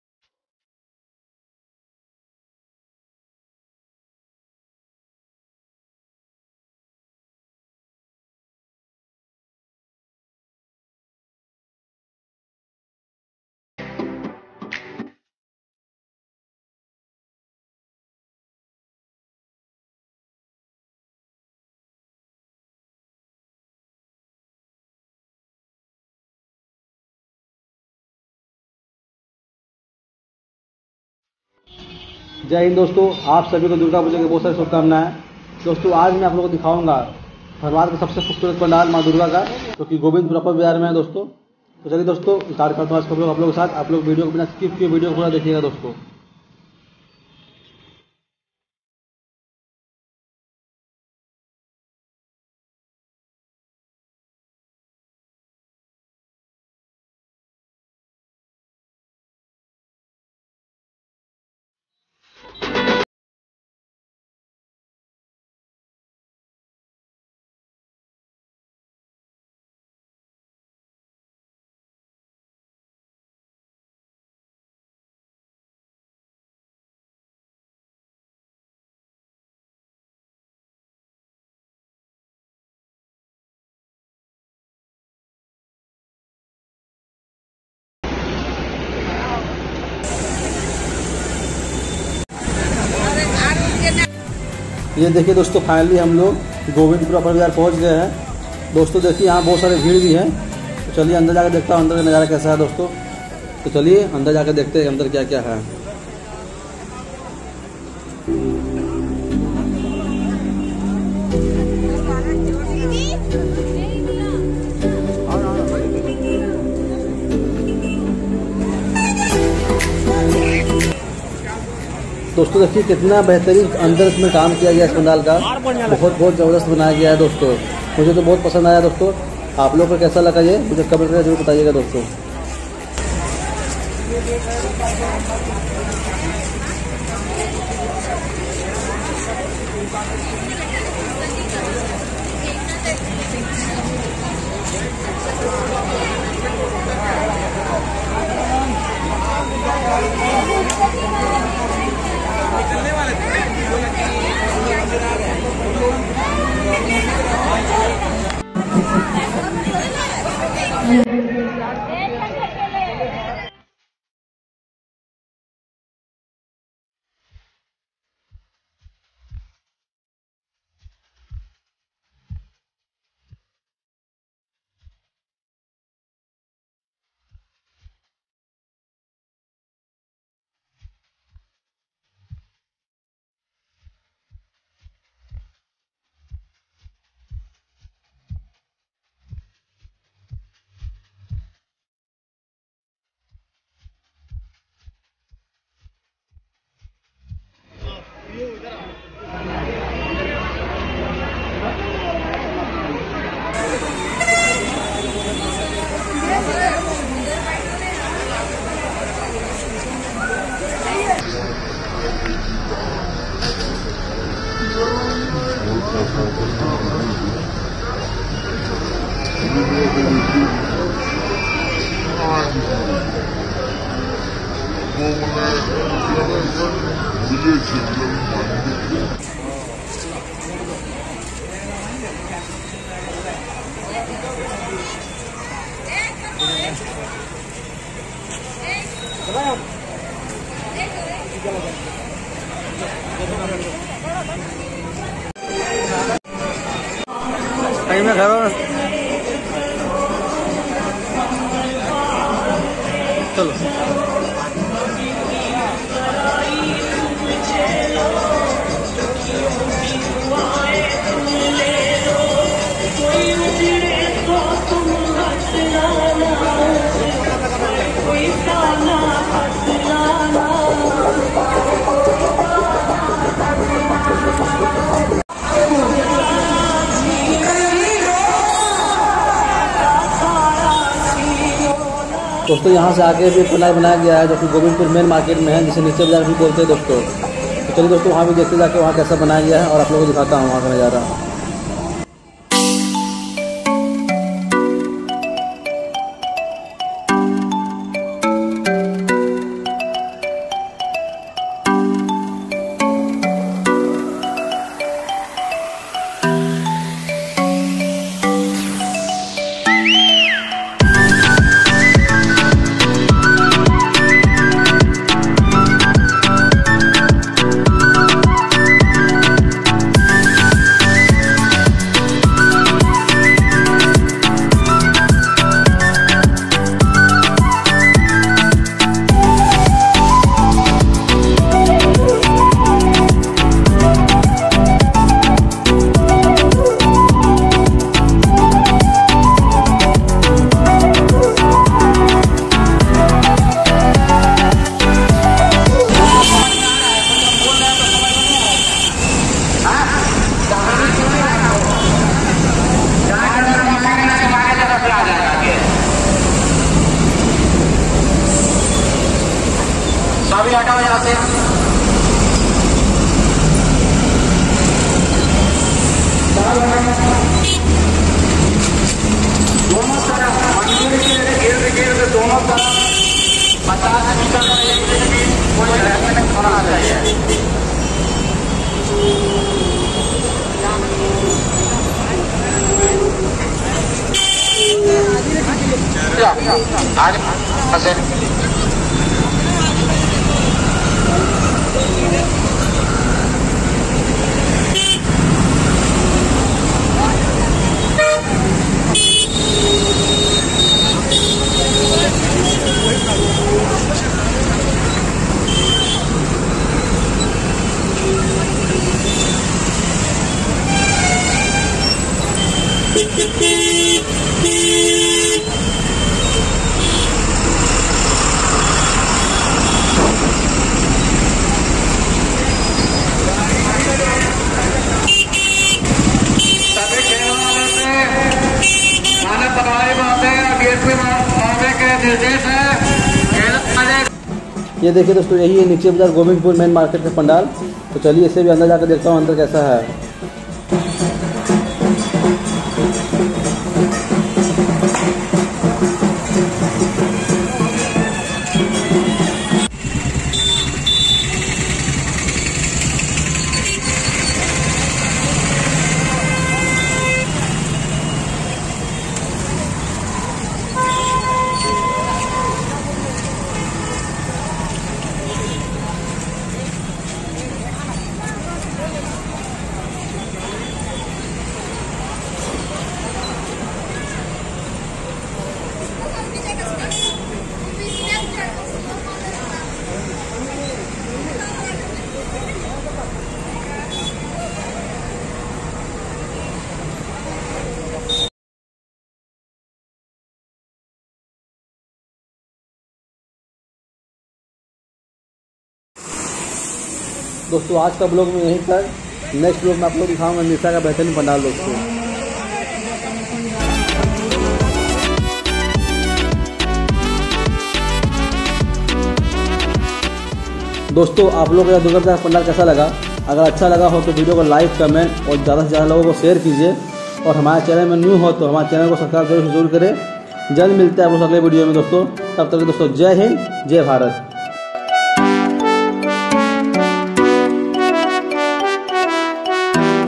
Dhanbad Durga Puja Best Pandels In Govindpur Upper Bazar | गोविंदपुर का फेमस दुर्गा पूजा पंडाल धनबाद
DHANBAD DURGA PUJA BEST PANDELS IN GOVINDPUR UPPER BAZAR | धनबाद का सबसे फेमस दुर्गा पूजा पंडाल गोविंदपुर

        ((. RAJA HINDUSTANI VLOGS.))